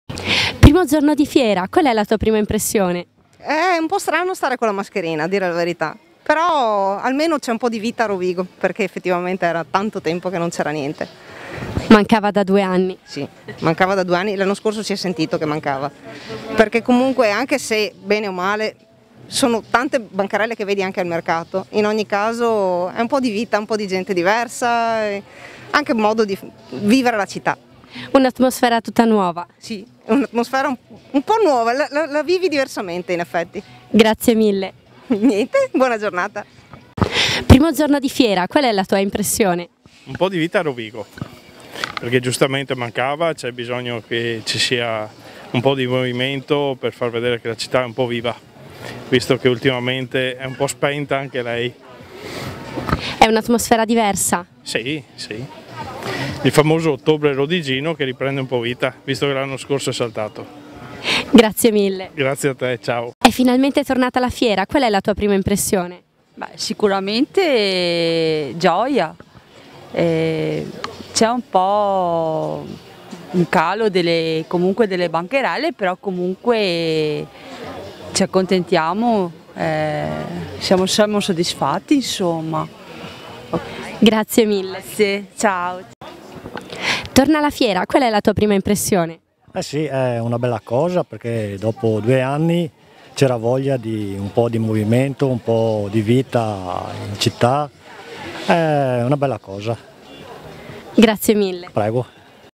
Finalmente è tornata la Fiera: e i protagonisti siete voi! - Le prime impressioni
Finalmente torna la grande Fiera d'Ottobre, a Rovigo. Ecco le prime impressioni dei Rodigini, ma anche dei tanti non rodigini che sono arrivati per la grande festa appena partita!